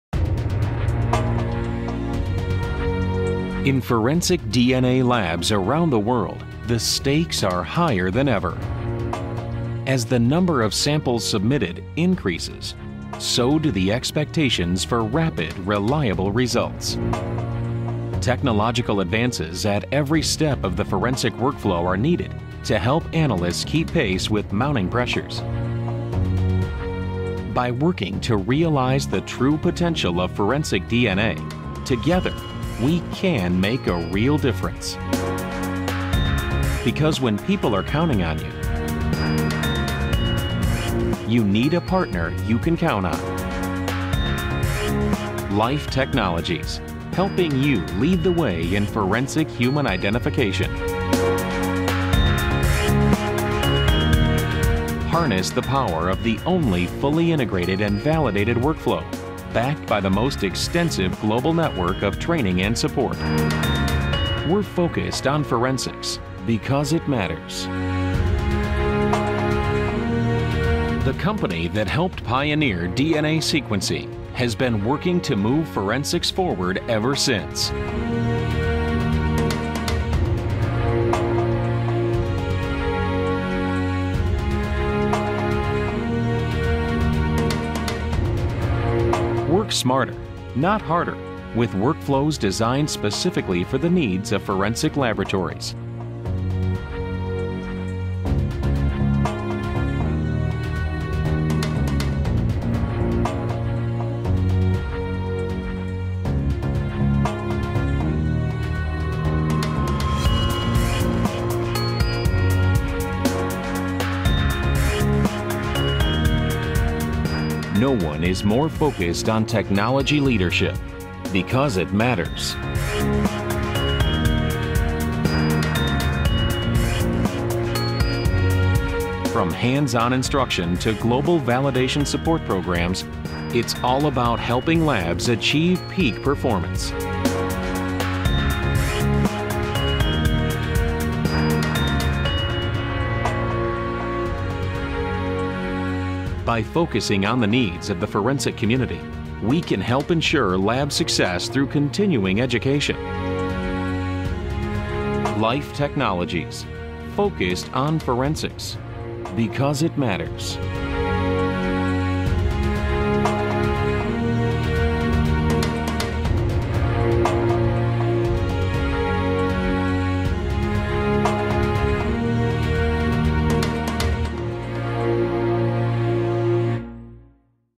DNA Forensics from Life Technologies -- Because It Matters
In forensic DNA labs around the world, the stakes are higher than ever. That's why Life Technologies remains focused on providing the most reliable and efficient solutions for human identification, backed by the most extensive global network of training and support. Because when people are counting on you, you need a partner you can count on.